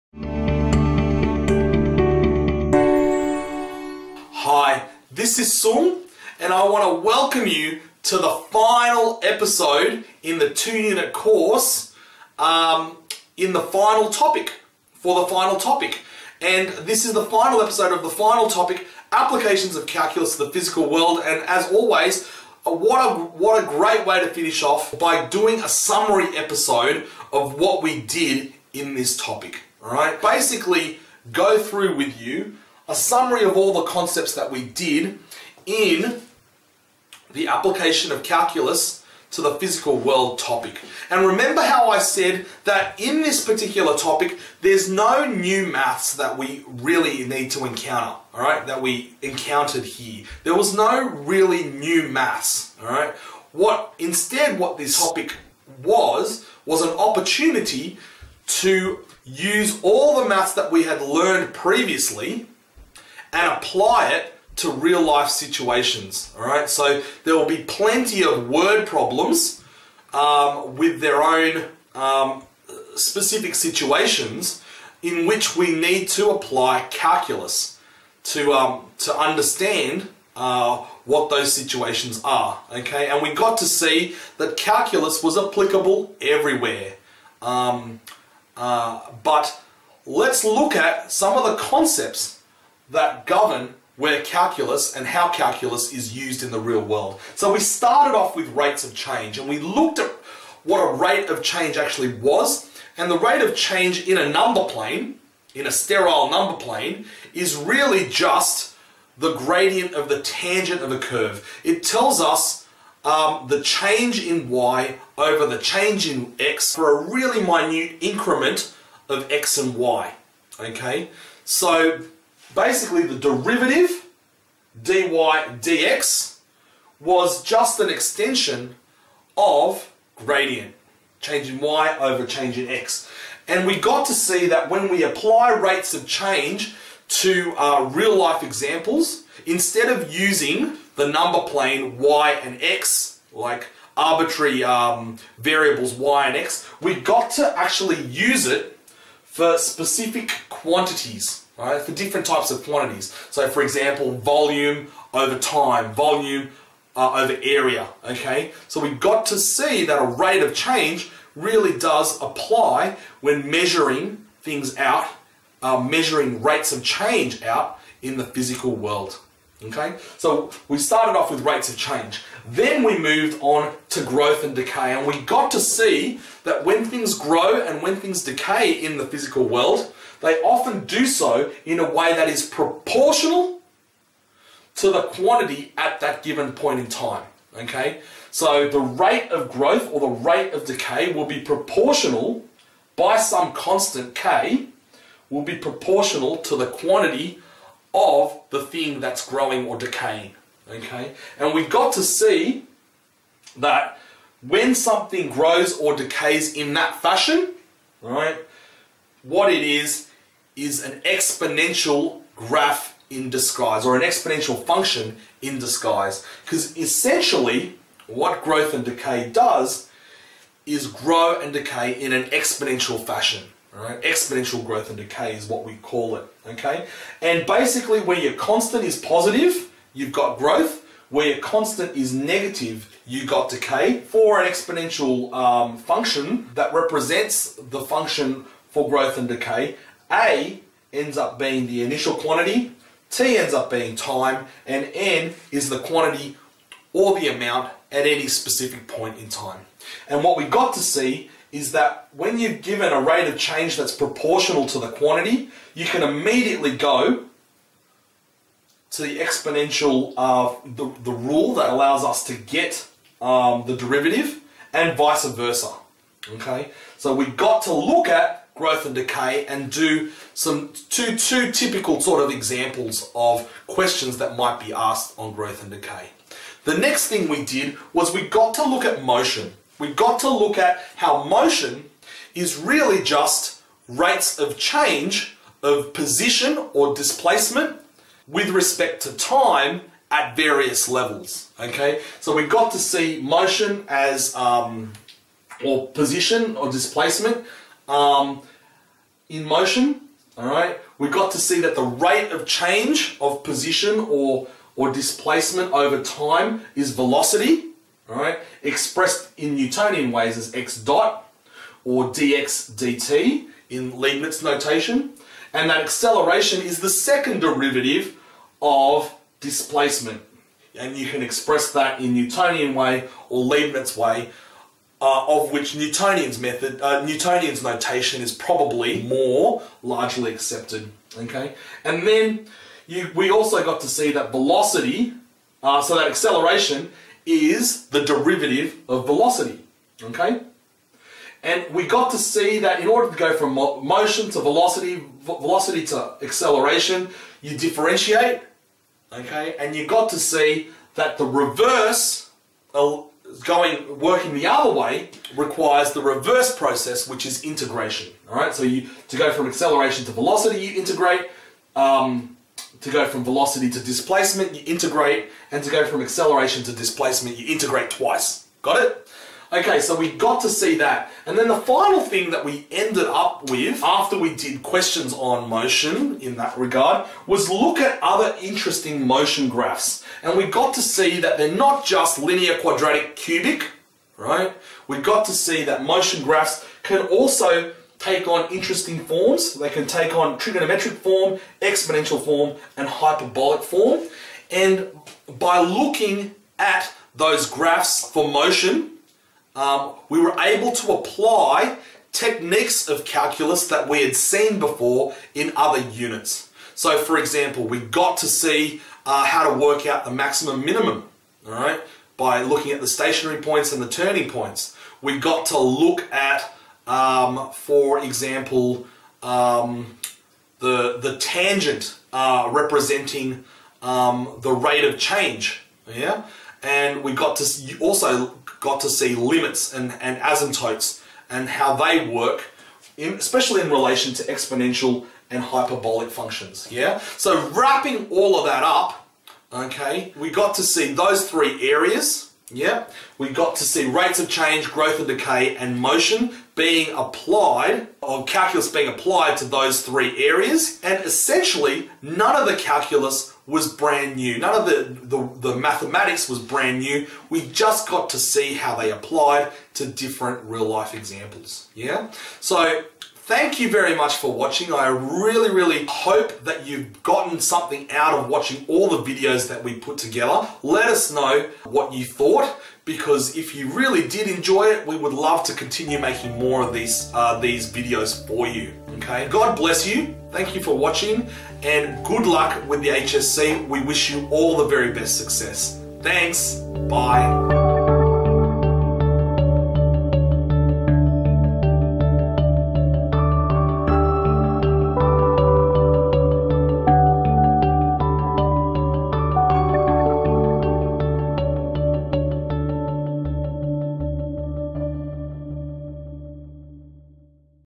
Applications of Calculus to the Physical World Summary - Sky Academy
This video is about the final episode of the 2 Unit course to help students learn and study Maths online effectively. In this video, Seung talks about the Rates of Change, Growth and Decay, Other Interesting Motion Graphs. This Application of the Physical World summary video also discusses about Displacement, Velocity and Acceleration.
SKY Academy Makes Maths Easy!

LET’S CONNECT!